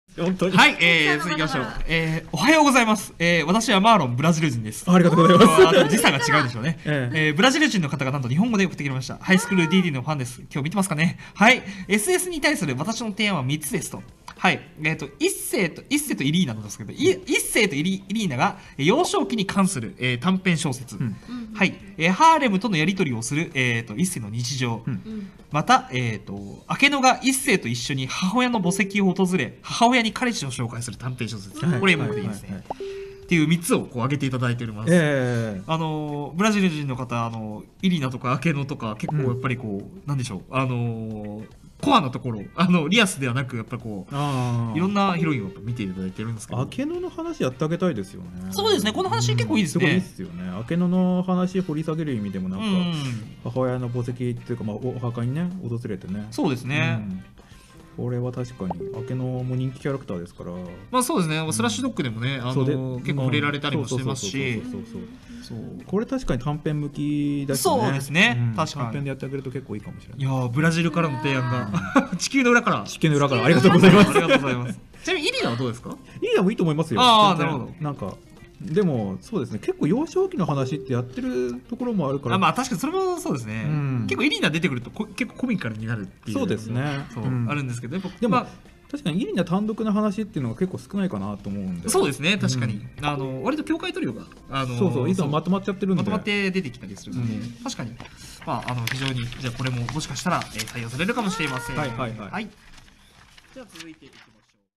Ichiei Ishibumi respondendo a Sugestões de fãs Brasileiros da DxD FC
Em uma live lendo sugestões de fãs para SS na novel de DxD, Ishibumi acabou lendo as sugestões de nossos adms Ray e Mitsuya. Muito legal ver isso!